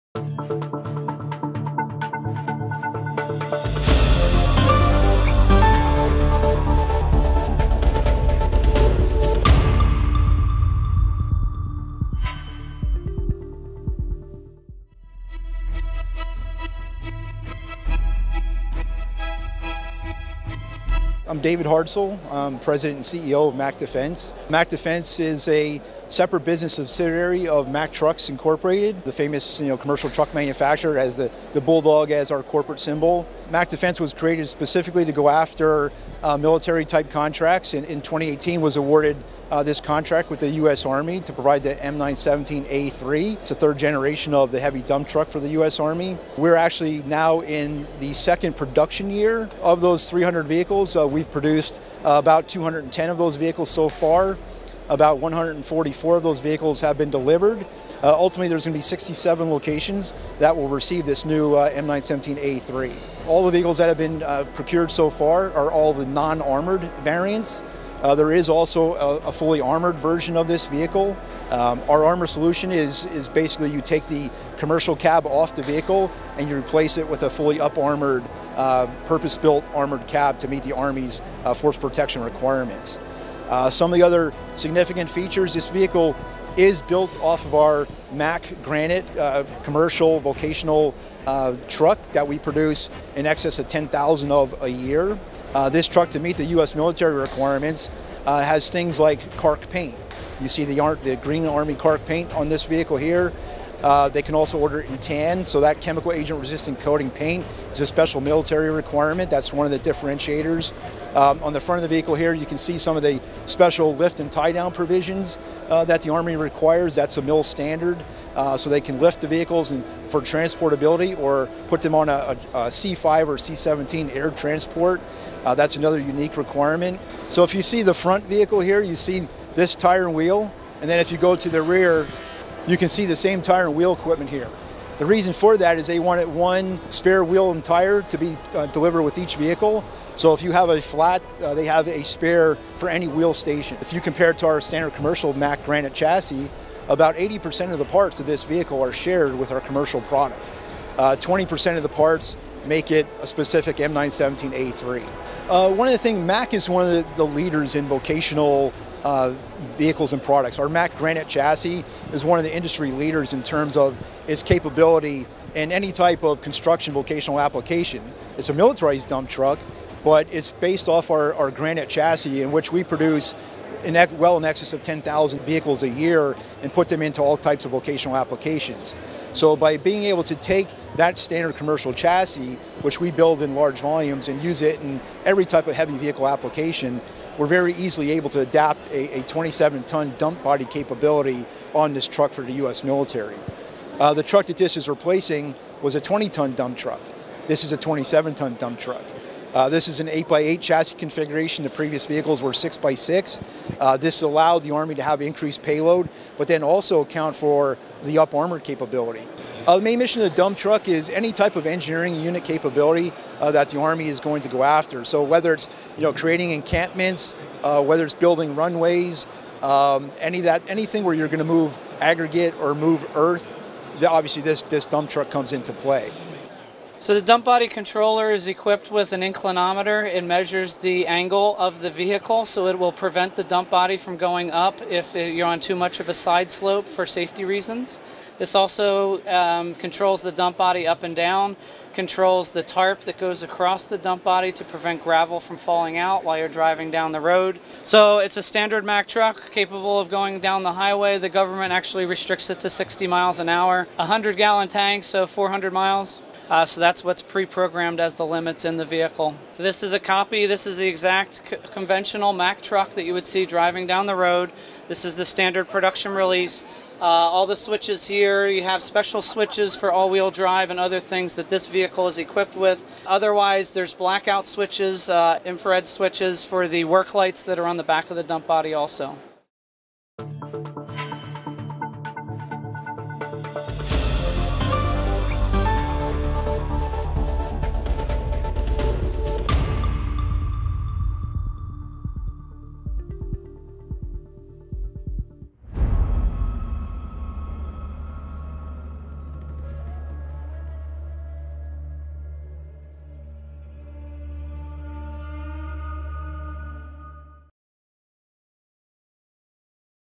Discover Mack Defense M917A3 latest generation of Heavy Dump Truck for US Army AUSA 2022 News
The American company Mack Defense presents the production version of its M917A3 Heavy Dump Truck at AUSA 2022, the Association of United States Army defense exhibition that takes place in Washington D.C. from 10 to 12 October 2022. The M917A3 is now in service with the U.S. Army.